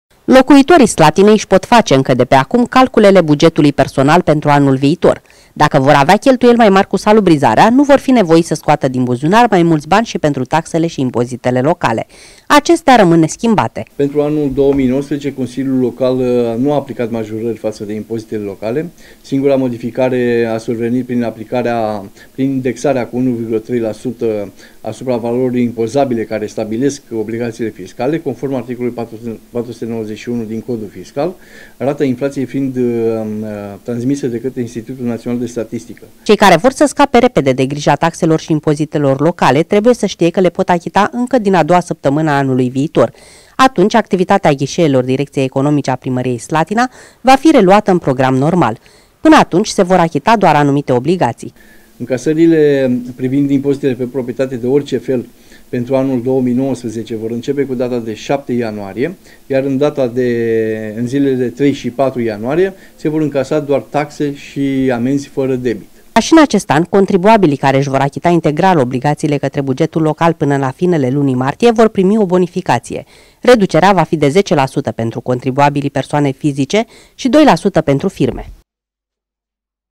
Taxele și impozitele locale rămân neschimbate pentru slătineni
Slătinenii au parte de vești bune din partea administrației locale, la finele acestui an. Ei vor trebui să achite către municipalitate, în 2019, aceleași taxe ca și anul acesta. Cei care vor să scape repede de grija datoriilor către bugetul local o pot face începând cu 7 ianuarie. Administrația slătineană acordă și anul viitor bonificația de 10% pentru persoane fizice și 2% pentru firme, dacă își achită integral dările până la finele lunii martie.